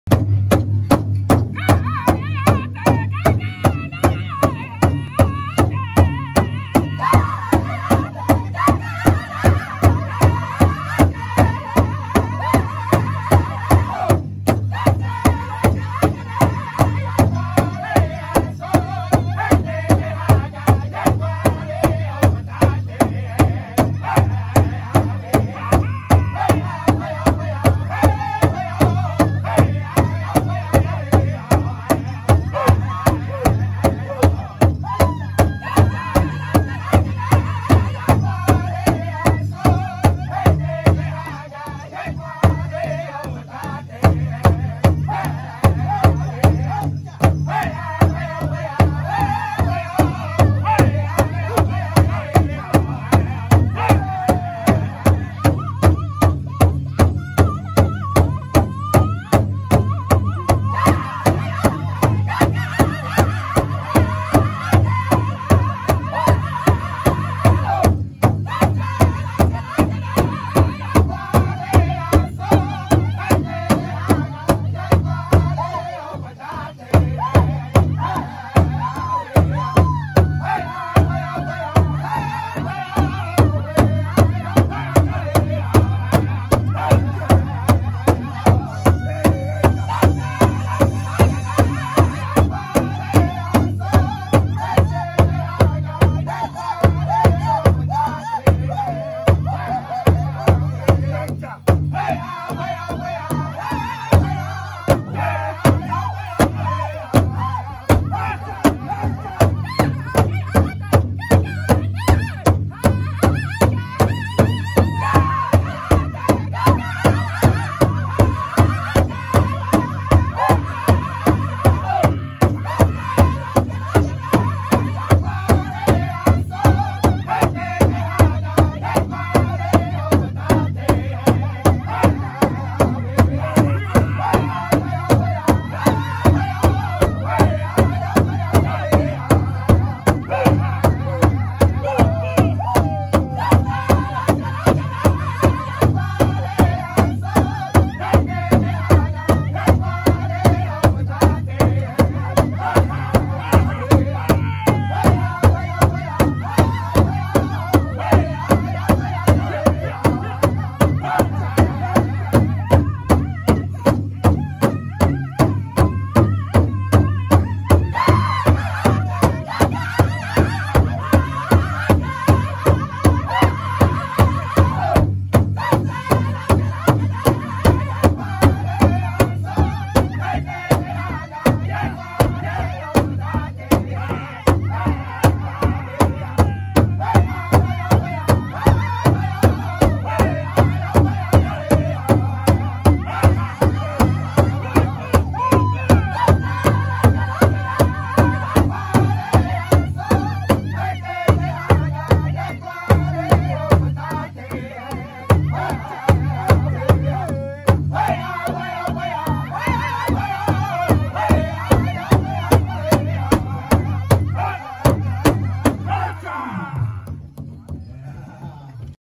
War Paint @ Rama Powwow 2022
War paint jamming an intertribal Saturday night live!